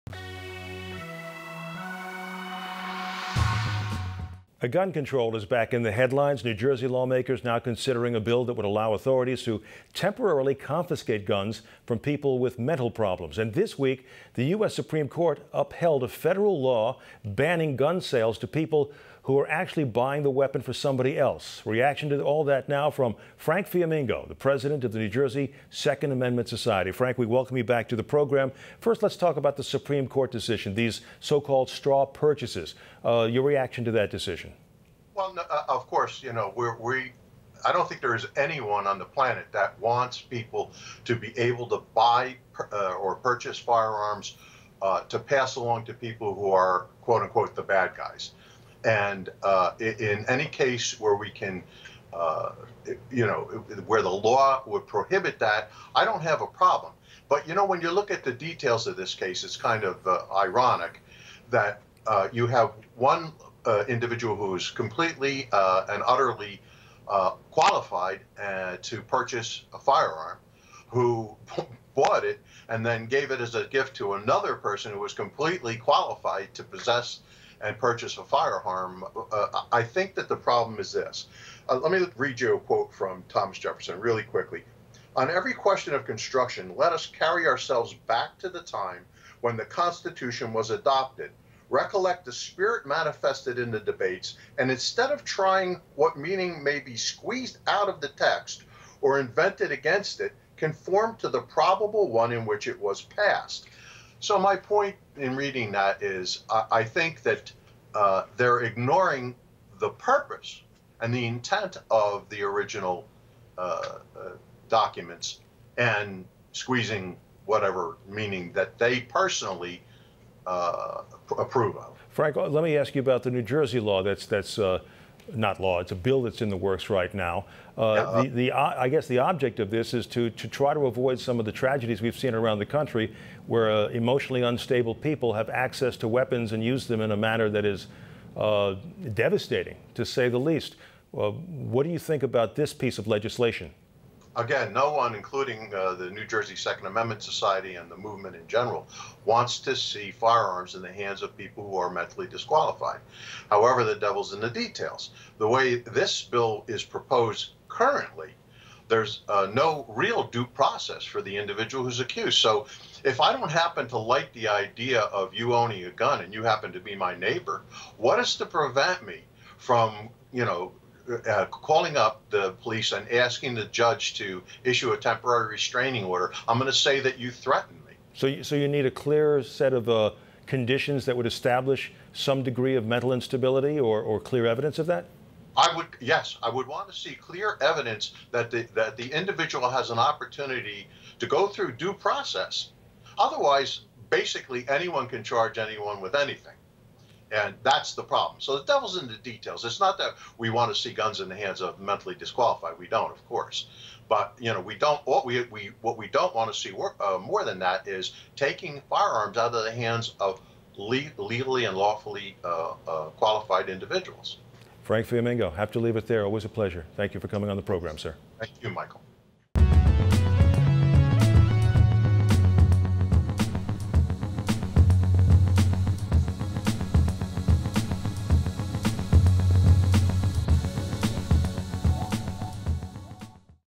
NJ Second Amendment Society Pres.: Gun Violence Bill Should Include Due Process
NJ Second Amendment Society President Frank Fiamingo says that a proposed bill in the state to take firearms away from mentally unstable individuals needs to include due process to present clear evidence a mental illness exists. For more New Jersey news, visit NJTV News online at njtvnews.org